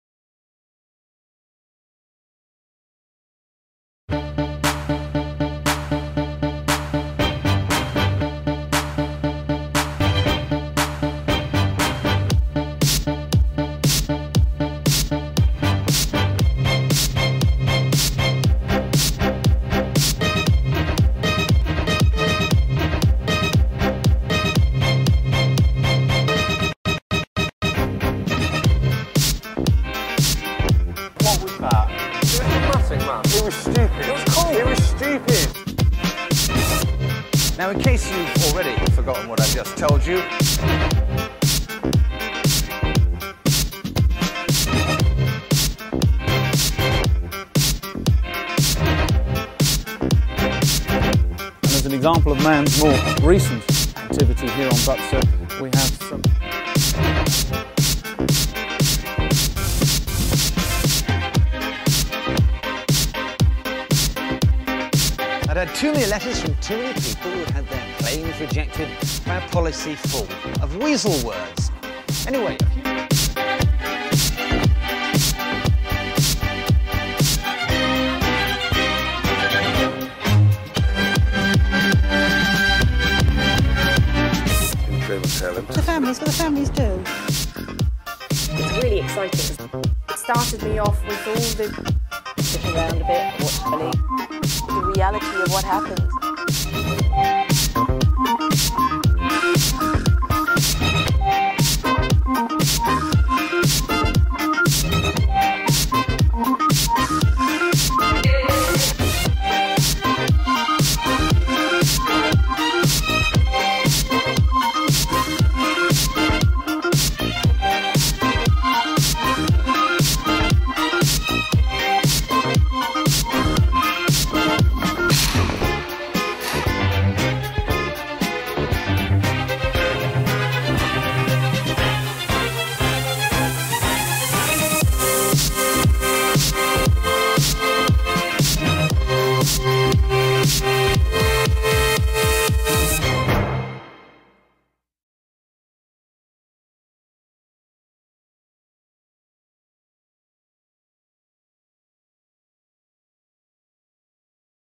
Dom Archer freelance cameraman / editor showreel 2009
Freelance Cameraman Showreel. Featuring shoots from Honda, Peugeot, BP, The Royal Navy, NATO, NATS, Vodafone and more.